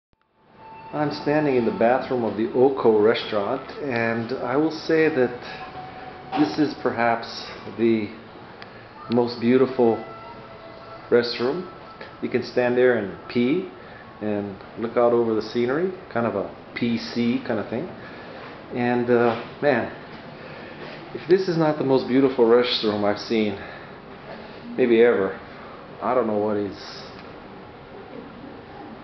Nicest restroom in the world
This is the restroom of the Oko restaurant in Bangalore, India.